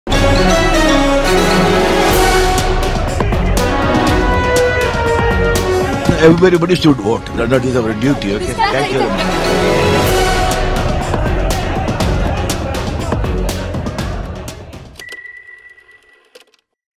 QuintHindi: चेन्नई में सुपरस्‍टार रजनीकांत ने डाला वोट
चेन्‍नई के स्‍टेला मारिस कॉलेज बूथ जा कर सुपरस्‍टार रजनीकांत ने अपना वोट डाला. वोट डालने को हमारा कर्तव्‍य बताते हुए सभी को वोट डालना की अपील भी की.